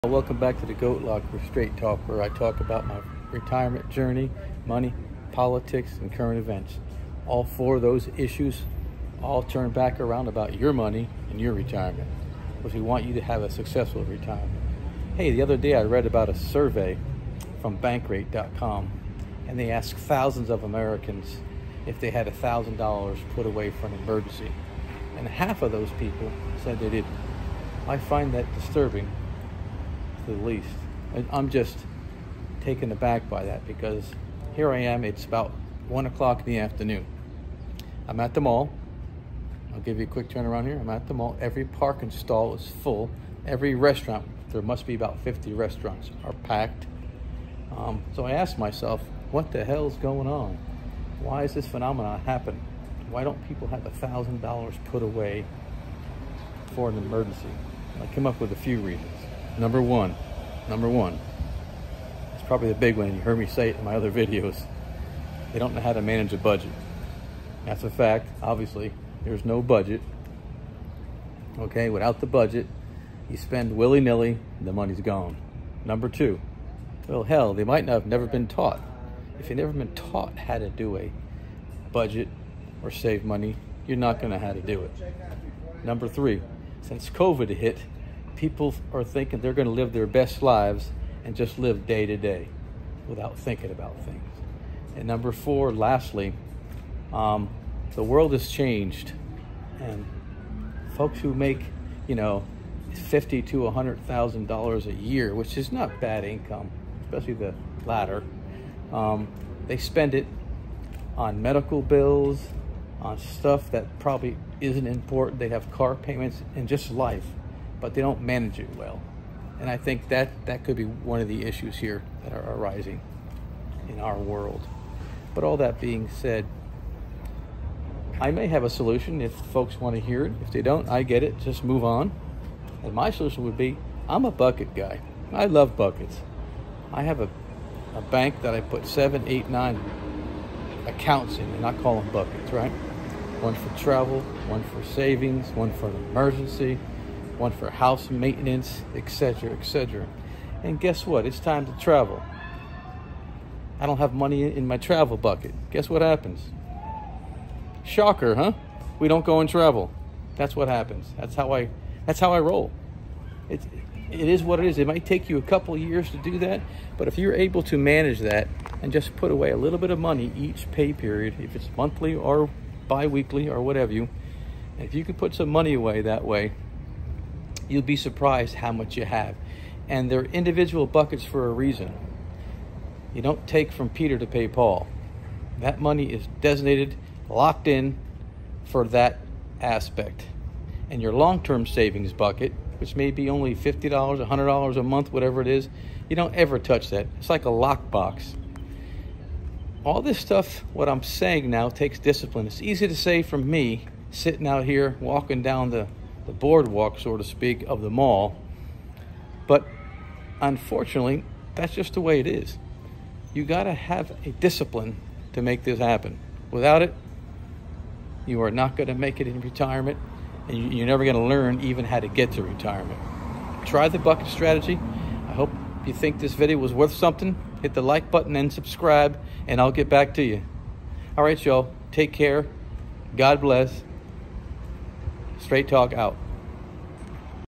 I am a Bucket type a guy: Goat Locker Straight Talk with Mike & Tony/10 June 2023
Mike discusses his thoughts on why half of Americans do not at least 1,000.00 in an Emergency fund